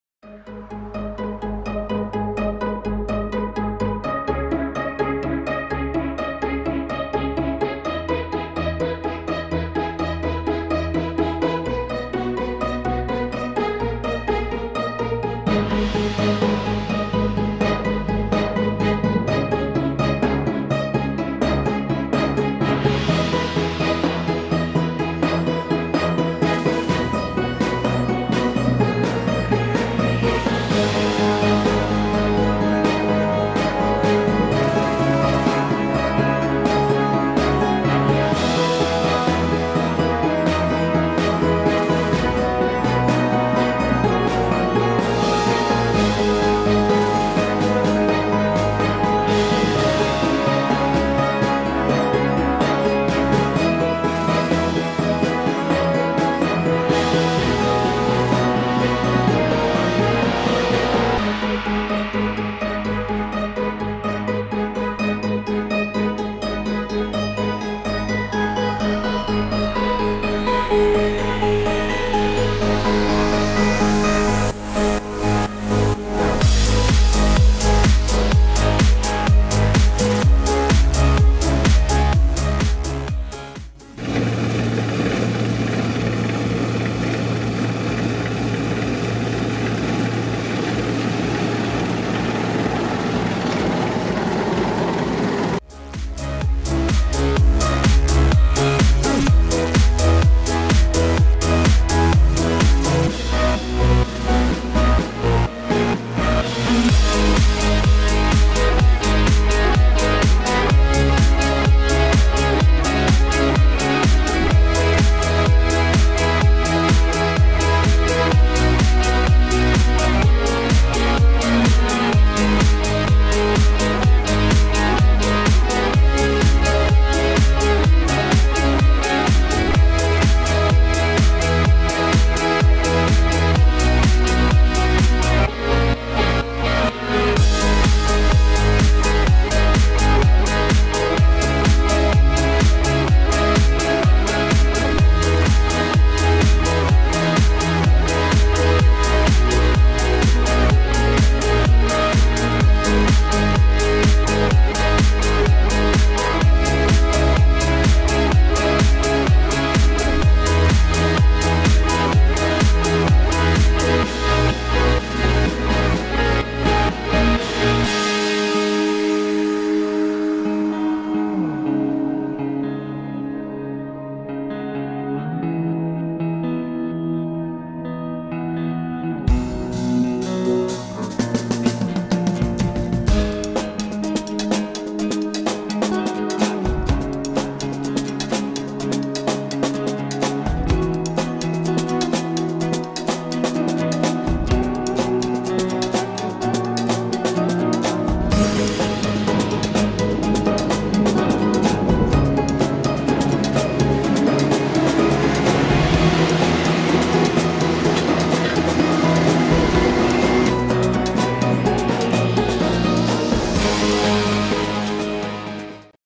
Harvest / Moisson - 2016 - NEW Massey ferguson Centora 7380 - Sarl PRAUD DT
Aujourd'hui c'est moisson de blé en Loire-atlantique avec cette magnifique Massey ferguson de l'entreprise Sarl Praud DT de Saint-Gildas-Des-Bois. Merci à eux.